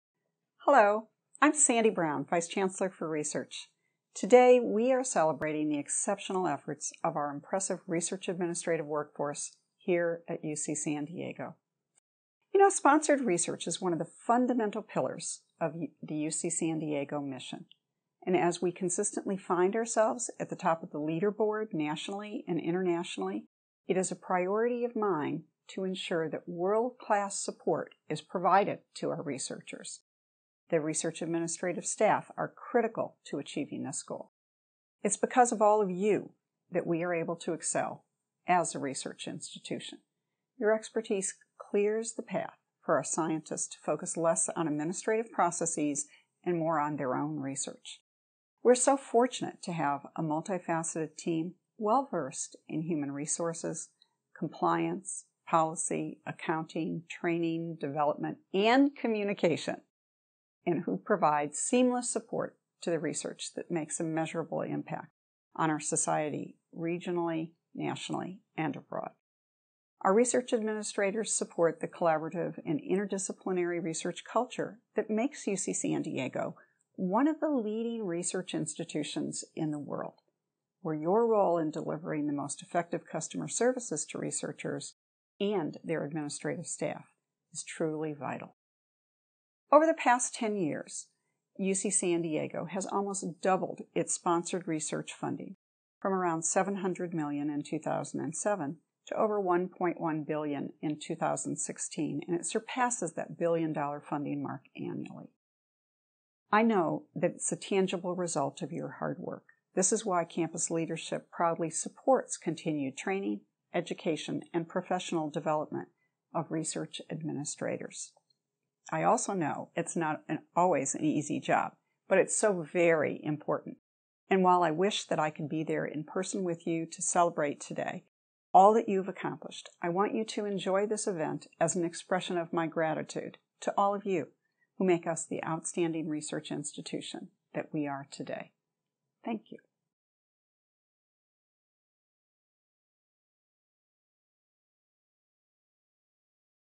UC San Diego Vice Chancellor Dr. Sandra Brown | Research Administrator Appreciation Day 2017
UC San Diego Vice Chancellor of Research Affairs, Dr. Sandra Brown, sends a message for the research administrators that attended the annual Research Administrator Appreciation Day.